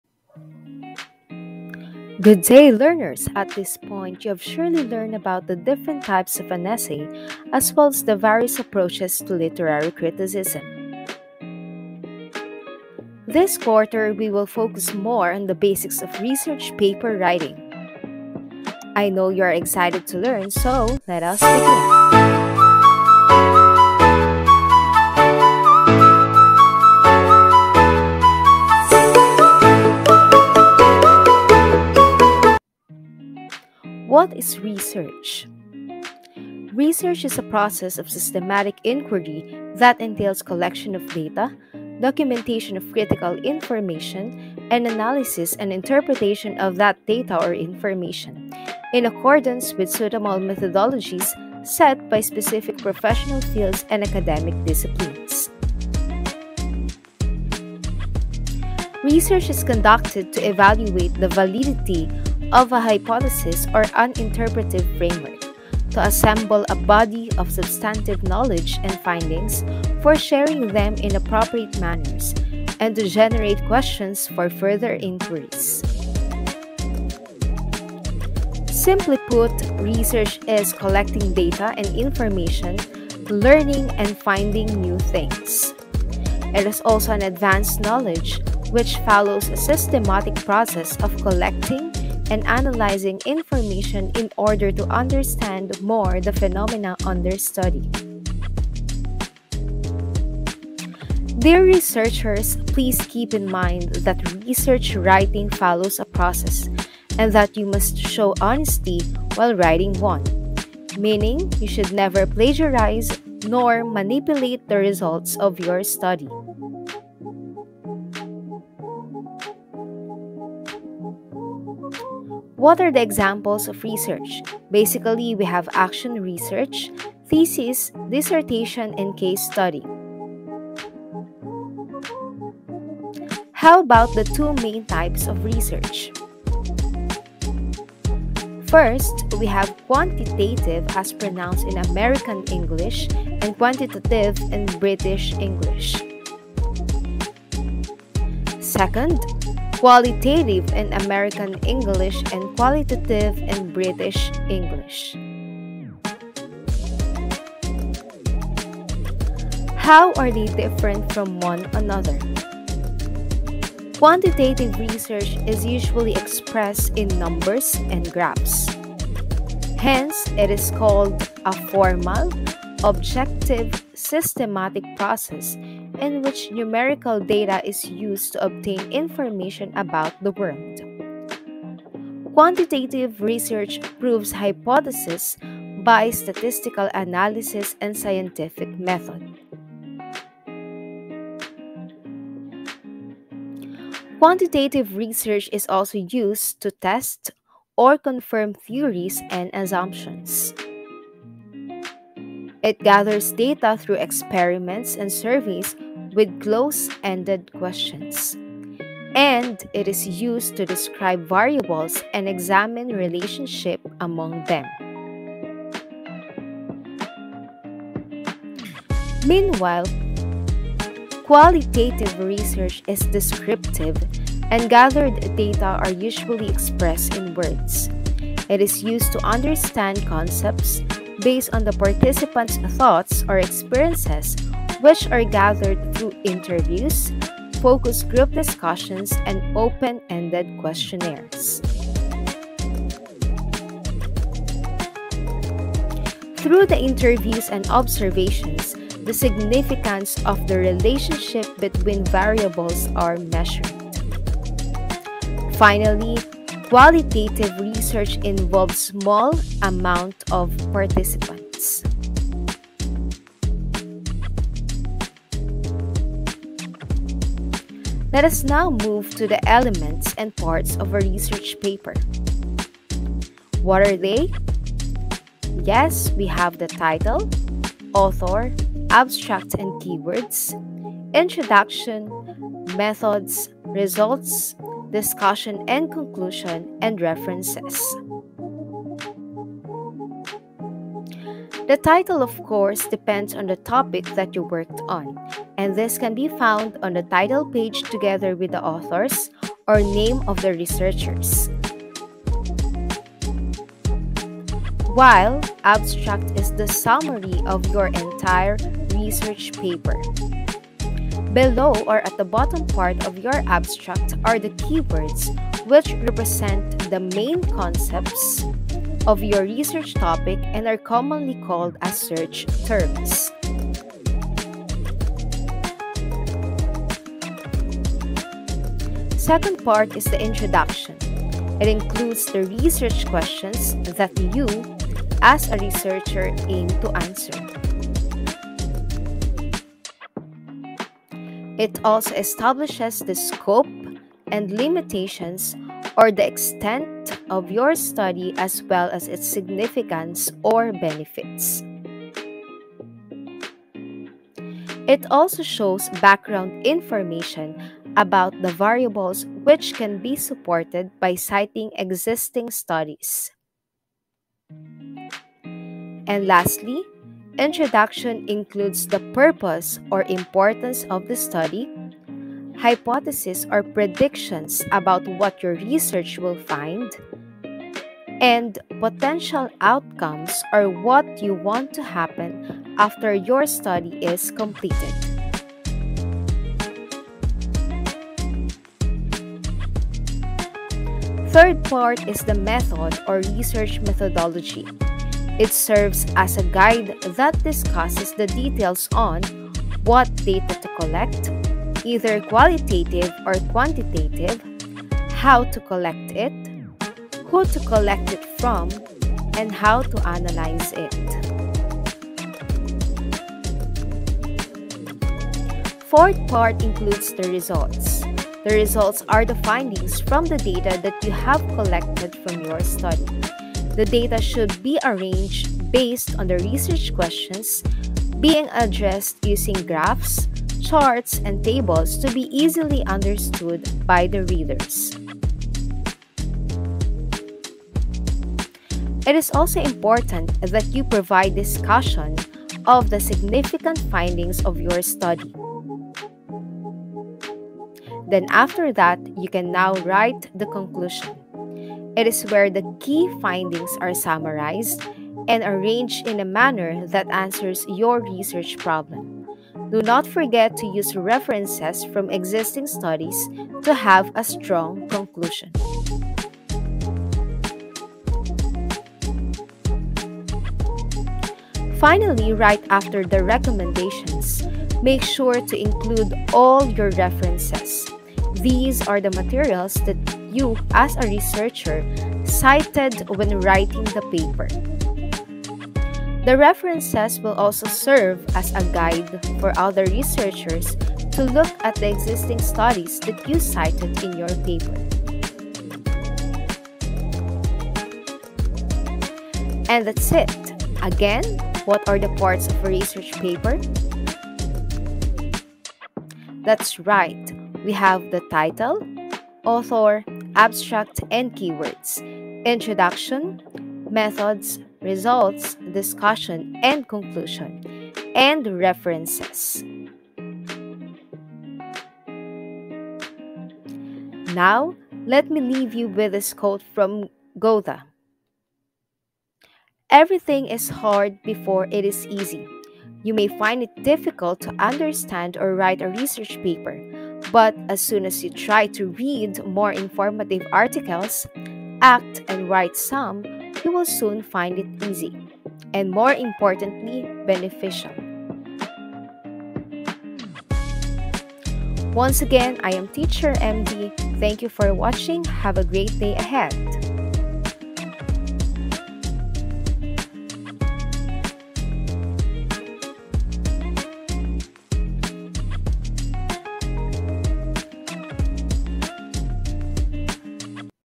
G10 Q4-Week 1: Research Paper and Its Parts
Learn with Teacher MD!
Understand what research is and determine all of its parts. Plus, get to know the difference between qualitative and quantitative research. Hurry! Watch now!

References:
ADM for English 10
G10 Learner's Packet in English-Week 2